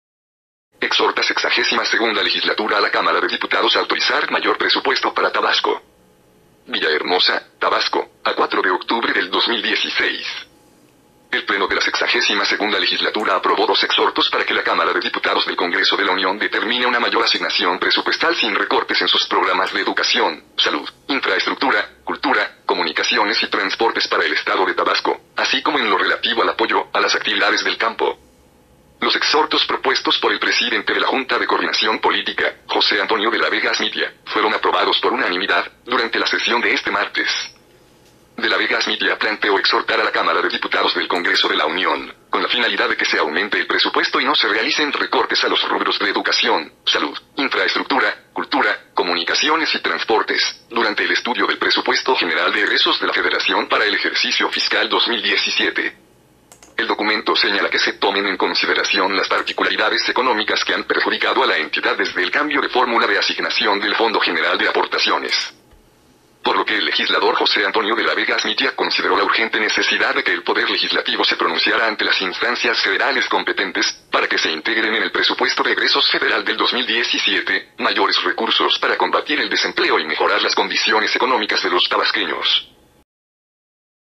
Exhorta LXII Legislatura a la Cámara de Diputados autorizar mayor presupuesto para Tabasco
Exhorta sexagésima segunda Legislatura a la Cámara de Diputados autorizar mayor presupuesto para Tabasco.

Villahermosa, Tabasco, a 4 de octubre del 2016.

El Pleno de la sexagésima segunda Legislatura aprobó dos exhortos para que la Cámara de Diputados del Congreso de la Unión determine una mayor asignación presupuestal sin recortes en sus programas de Educación, Salud, Infraestructura, Cultura, Comunicaciones y Transportes para el Estado de Tabasco, así como en lo relativo al apoyo a las actividades del campo.

Los exhortos propuestos por el presidente de la Junta de Coordinación Política, José Antonio De La Vega Asmitia, fueron aprobados por unanimidad, durante la sesión de este martes.

De La Vega Asmitia planteó exhortar a la Cámara de Diputados del Congreso de la Unión, con la finalidad de que se aumente el presupuesto y no se realicen recortes a los rubros de Educación, Salud, Infraestructura, Cultura, Comunicaciones y Transportes, durante el estudio del Presupuesto General de Egresos de la Federación para el ejercicio fiscal 2017.

El documento señala que se tomen en consideración las particularidades económicas que han perjudicado a la entidad desde el cambio de fórmula de asignación del Fondo General de Aportaciones.

Por lo que el legislador José Antonio De La Vega Asmitia consideró la urgente necesidad de que el Poder Legislativo se pronunciara ante las instancias federales competentes, para que se integren en el Presupuesto de Egresos Federal del 2017, mayores recursos para combatir el desempleo y mejorar las condiciones económicas de los tabasqueños.